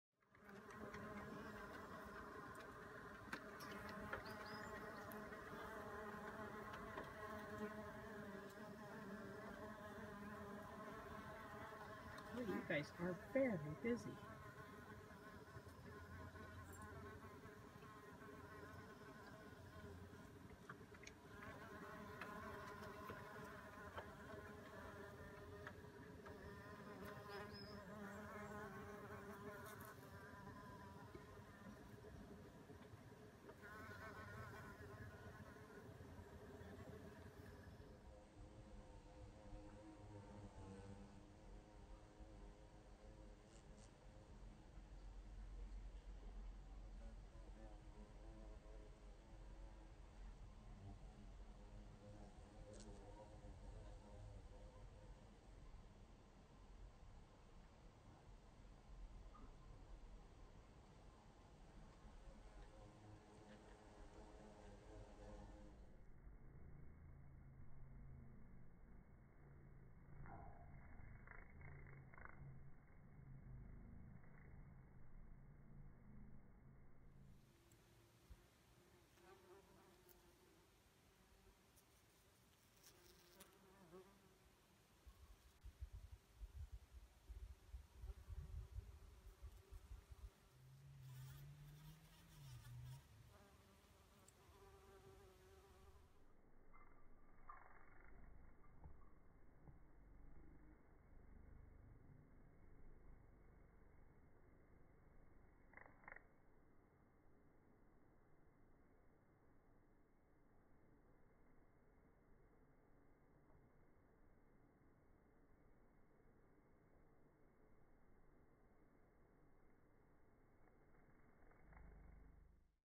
Mason Bees & More - April 2016
Some video of the mason bees (and a bumble bee) in our back yard on a warm spring day.